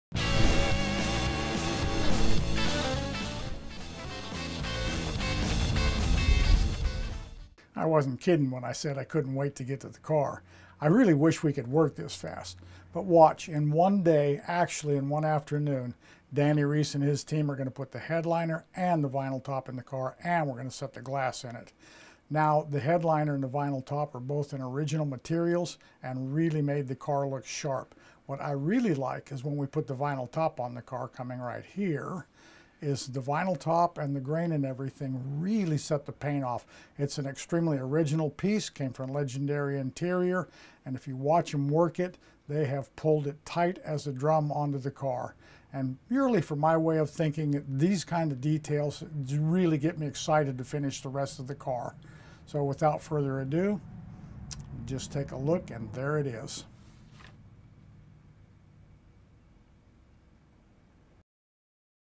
Body On A Chassis Car
In this video, it’s time to get the body on the Chassis. In one afternoon, the team puts the headliner and vinyl top on the car and also sets the glass in it.  Both the headliner and top are original materials which really make the car look sharp!